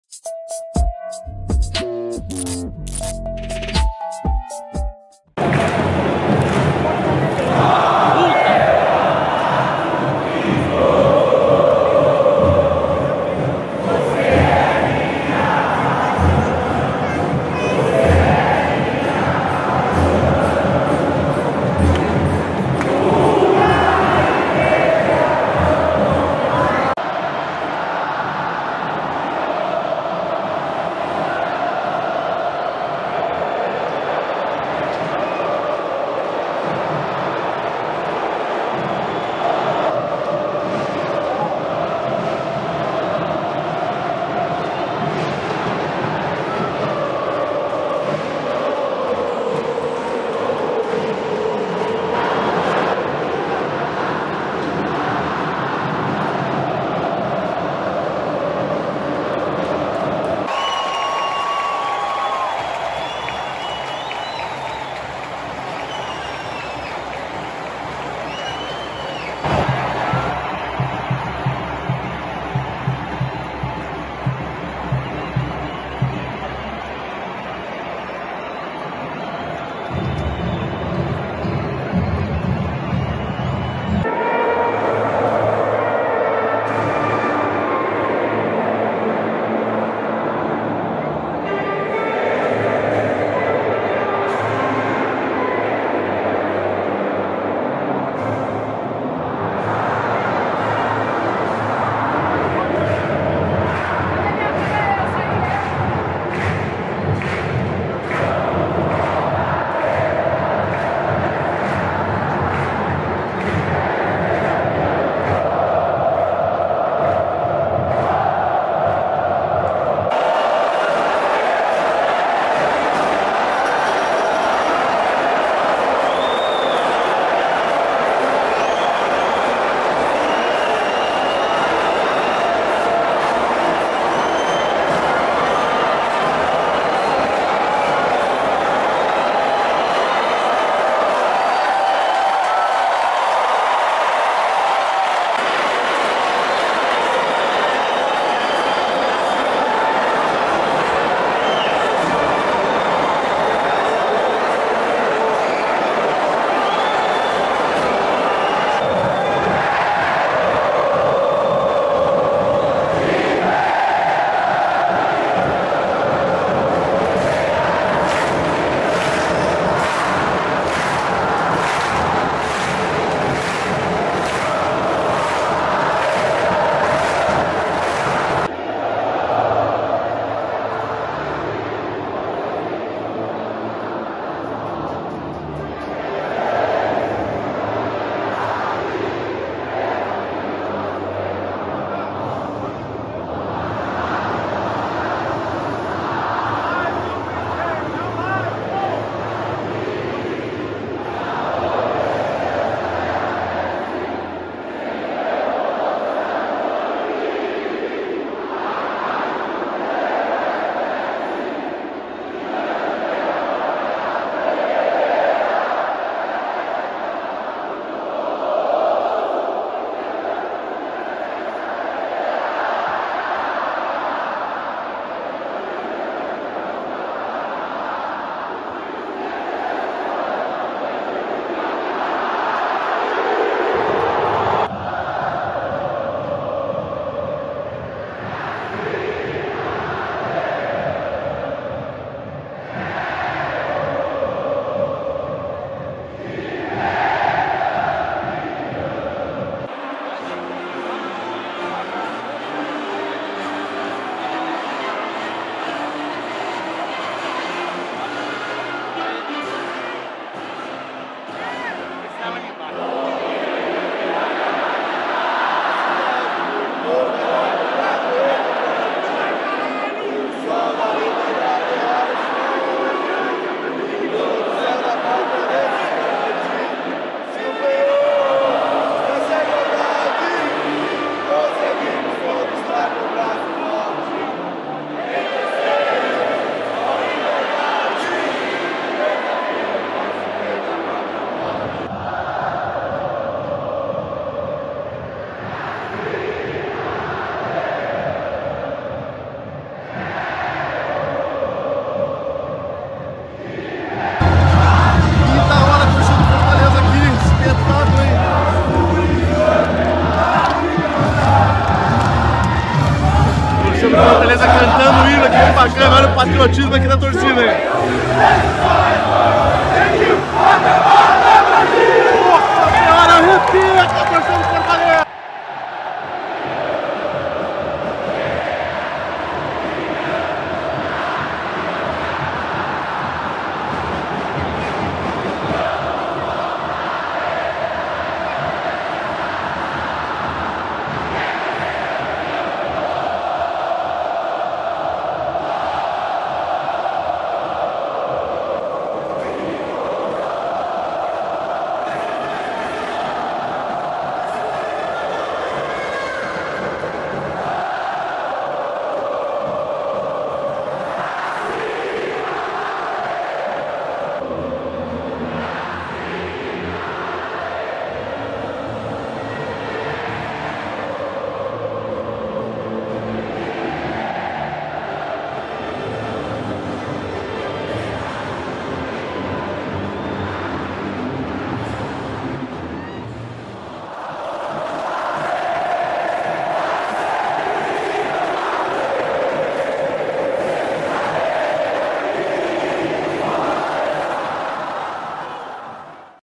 Esses foram TODOS os INCRÍVEIS MOSAICOS do FORTALEZA em 2023 - (Em Ordem Cronológica)
O vídeo de hoje é simplesmente TODOS os mosaicos de 2023
(Os mosaicos estão na ordem em que foram realizados)

SE INSCREVE SE AINDA NÃO FOR INSCRITO E VEM FAZER PARTE DA NAÇÃO

TÓPICOS
00:00 - Abertura
00:10 - Savana tricolor
00:27 - Tricolor é nós
01:00 - Juntos somos Fortaleza
01:24 - O Destino está chamando
02:00 - De Pai para filho
02:58 - Mega mosaico de Bandeirinhas
04:10 - Um clube, um sonho
04:52 - Leão permanente
05:04 - Final da Sulamericana
05:25 - Mega Mosaico de Led
06:04 - King Lion